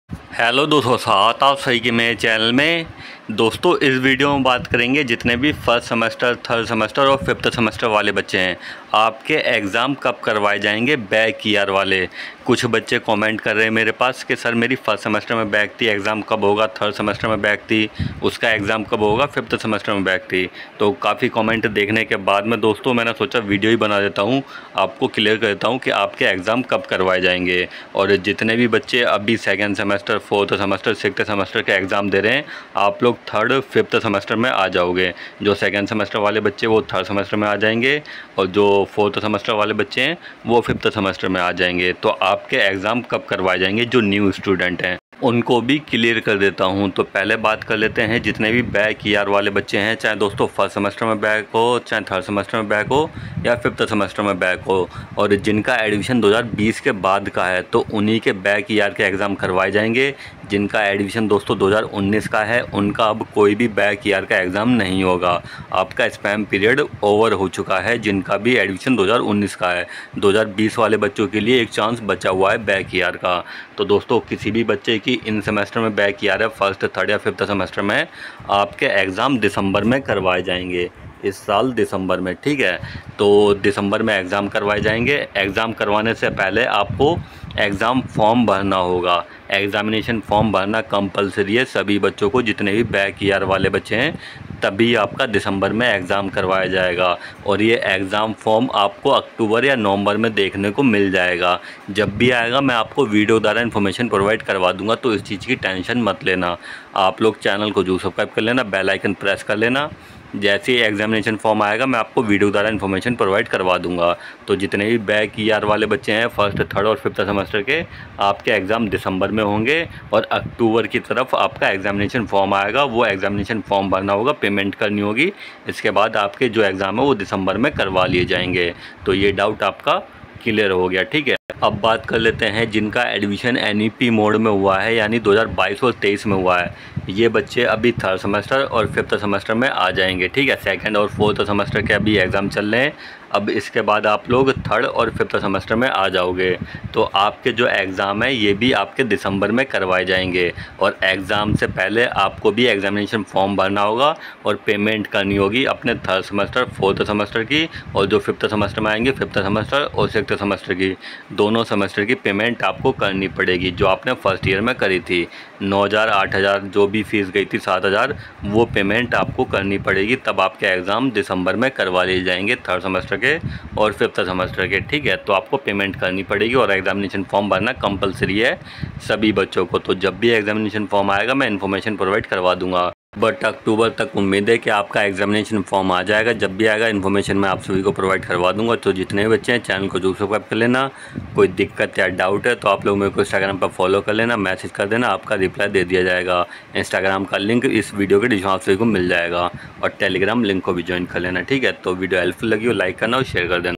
DU SOL 1st & 3rd & 5th Semester Fail/Back/ER Exam Dates and Examination Form 2024
1st & 3rd & 5th semester back, fail, ER Exam kab hoge?

_____________Instagram_____________________

👉___________Telegram Group___________